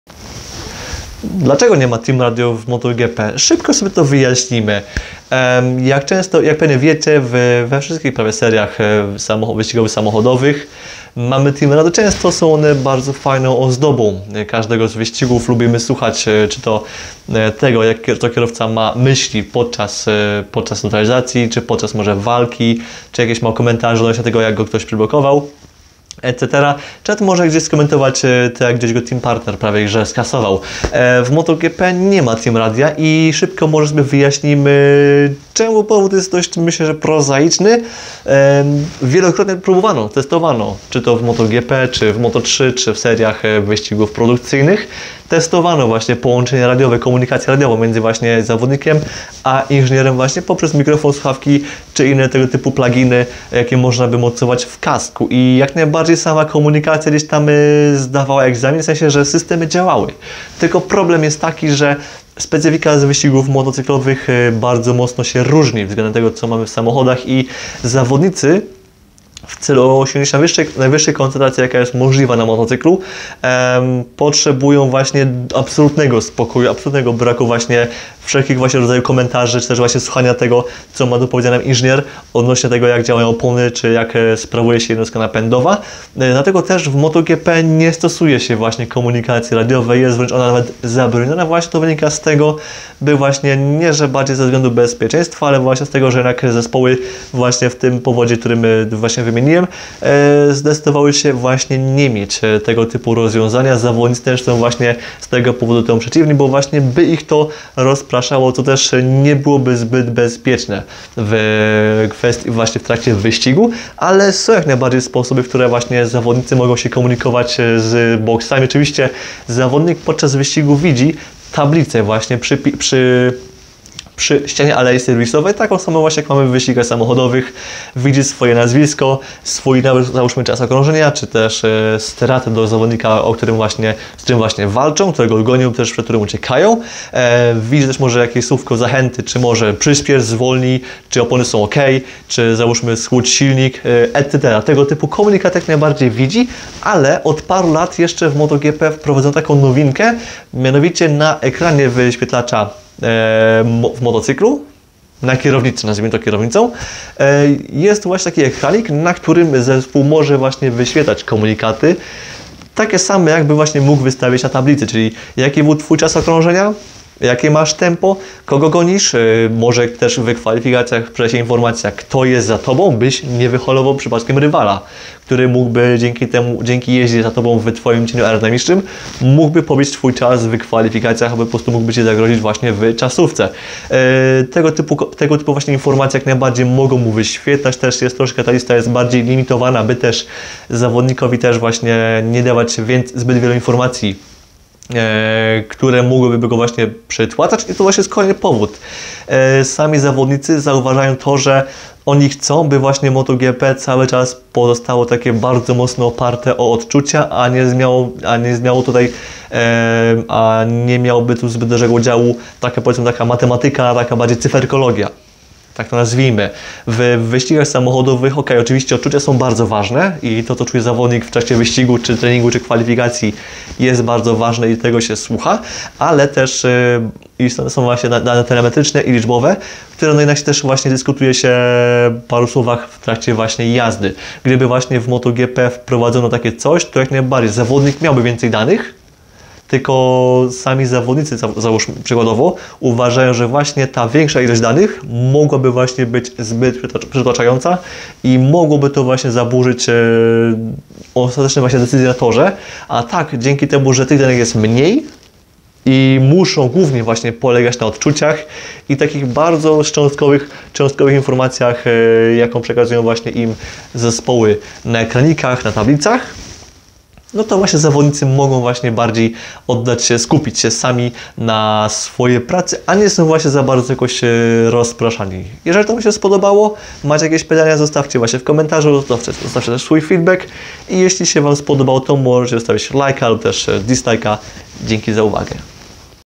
Dlaczego nie ma Team Radio w MotoGP i czy jest szansa na jego wprowadzenie?
f1 motogp f1, motogp, team radio, radio, team, Ferrari, motorsport